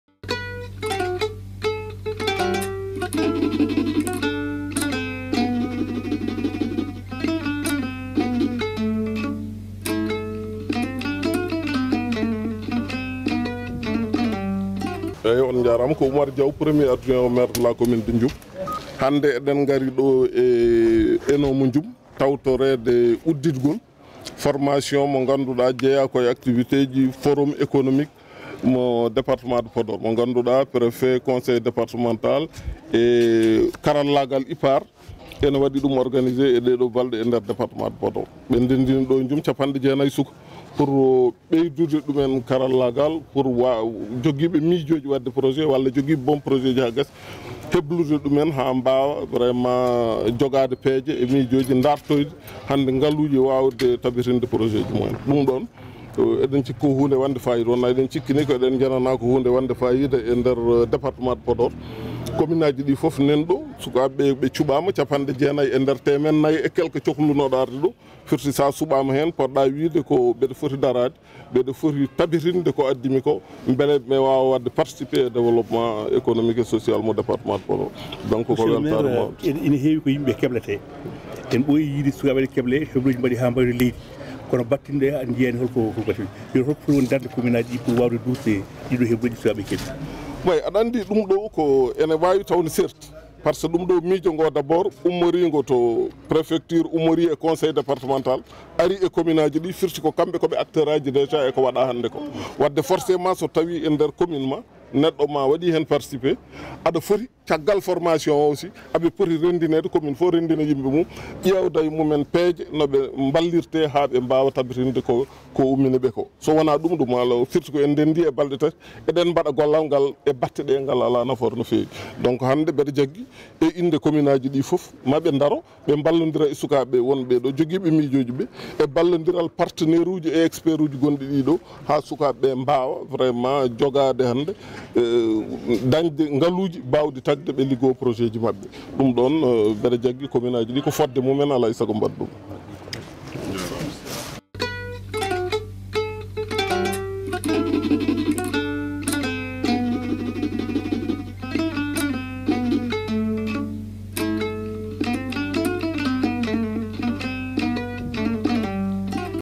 Oumar Diaw, Adjoint au maire de Ndioum au Forum économique de Podor: sur la formation des jeunes
Activez les notifications Merci d'avance